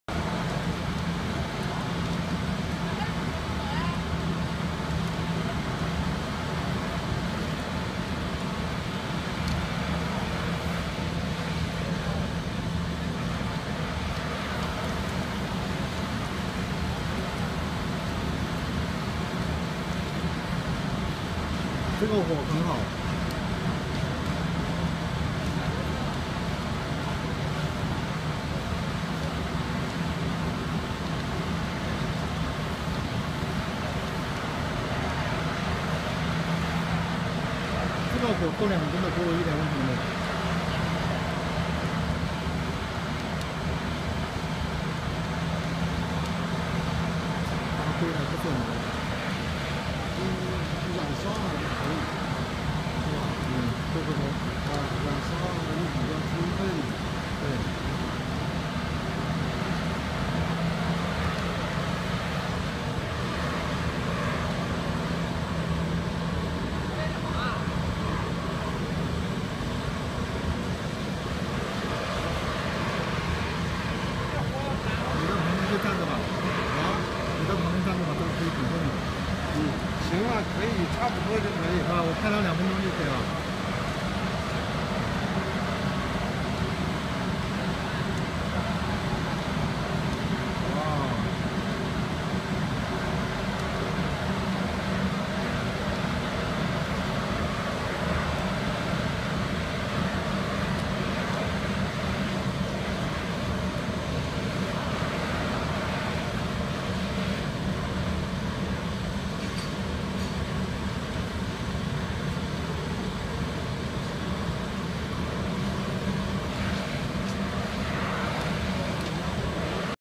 rice husk powder biomass burner for steam boiler
rice husk burner for steam boiler, the rice husk can from rice mill, vinasse, after crush the rice husk, they can be used for our biomass burner.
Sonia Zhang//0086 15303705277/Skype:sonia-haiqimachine